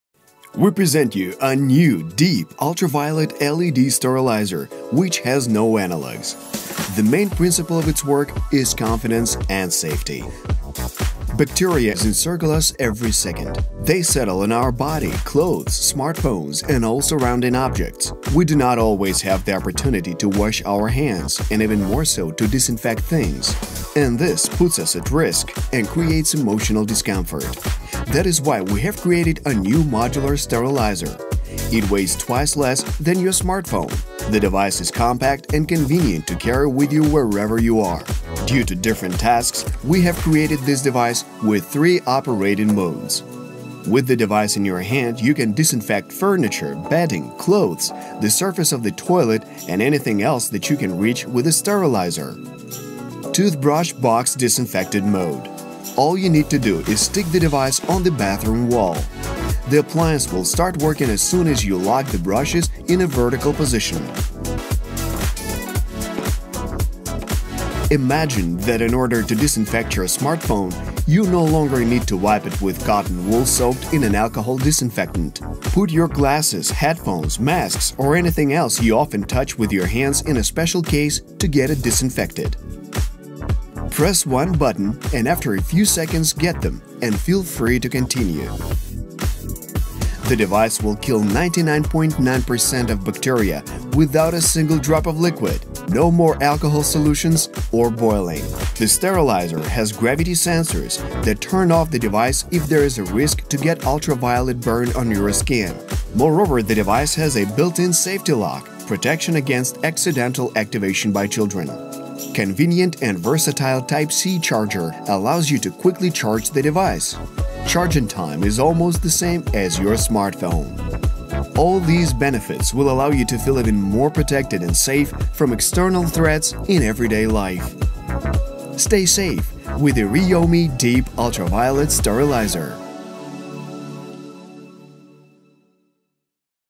Riomee Modular & Portable Deep UV Sterilizer
Riomee’s innovational Deep-UV Sterilizer is built in a modular manner, which means that it can be conveniently disassembled and assembled, and can serve a variety of purposes in different scenarios. Our product will become your number one mate in sterilizing everyday items wherever you are.

Whether you would like to disinfect your smartphone, newly purchased products or various segments of your home, the Riomee UVC-LED Deep-UV Sterilizer will help you accomplish all of these with ease.